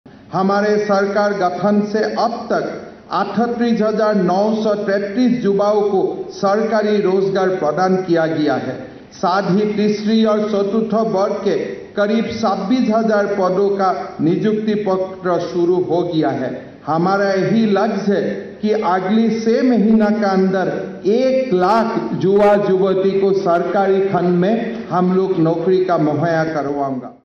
চৰকাৰী চাকৰি সন্দৰ্ভত মুখ্যমন্ত্ৰীৰ গুৰুত্বপূৰ্ণ ঘোষণা । আগন্তুক ছমাহৰ ভিতৰত হ’ব ১ লাখ নিযুক্তি
চৰকাৰী চাকৰি সন্দৰ্ভত মুখ্যমন্ত্ৰীৰ গুৰুত্বপূৰ্ণ ঘোষণা
আগন্তুক ছমাহৰ ভিতৰত হ’ব ১ লাখ নিযুক্তি